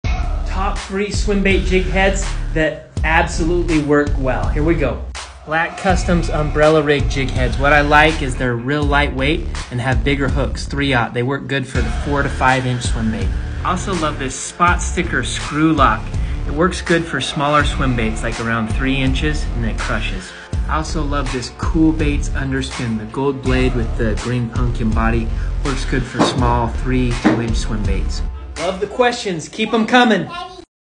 Top 3 Swimbait Jig Heads Bass Fishing
In this video I walk through the Top 3 swimbait jig heads that will help you catch a lot more fish on swimbaits. I see this a lot as a pro bass guide. A swimbait is one of the best lures to catch bass on, but you got to have the right jig head to match your bait. Use these tips next time you go out.